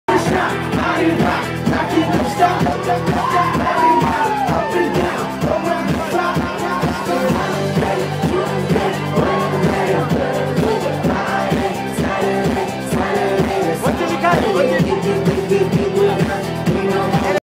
DJ Ranjit Raju DJing aboard the Captain Cook Cruise Fiji One off Port Denarau in Fiji
DJ Ranjit Raju DJing aboard the Captain Cook Cruise Fiji One off Port Denarau in Fiji, the boat is shaking with all the jumping!